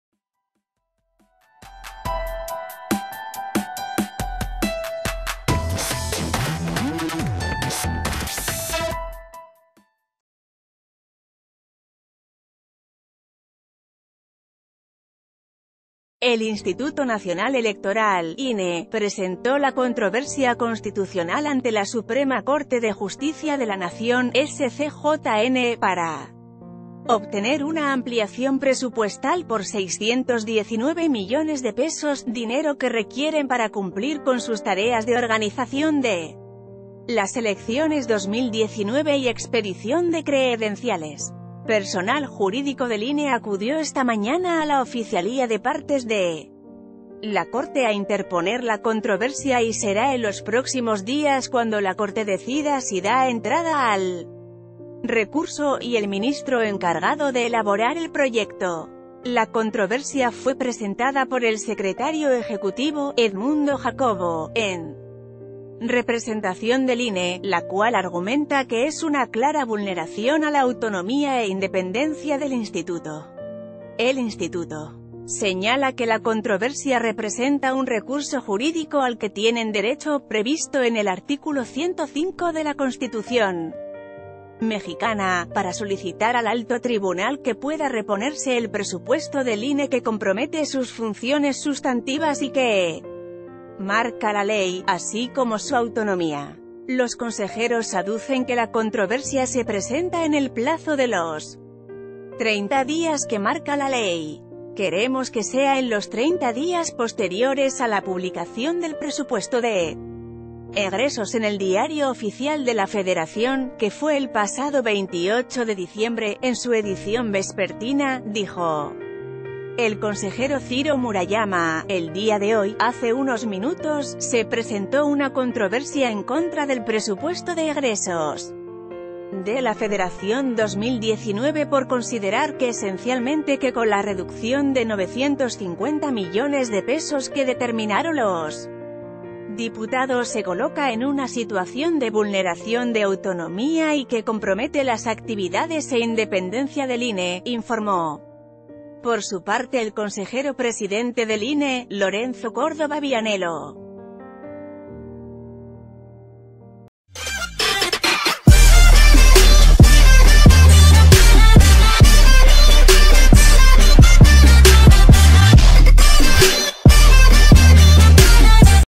INE presenta controversia ante la SCJN para obtener más presupuesto
El Instituto Nacional Electoral (INE) presentó la controversia constitucional ante la Suprema Corte de Justicia de la Nación (SCJN) para obtener una ampliación presupuestal por 619 millones de pesos, dinero que requieren para cumplir con sus tareas de organización de las elecciones 2019 y expedición...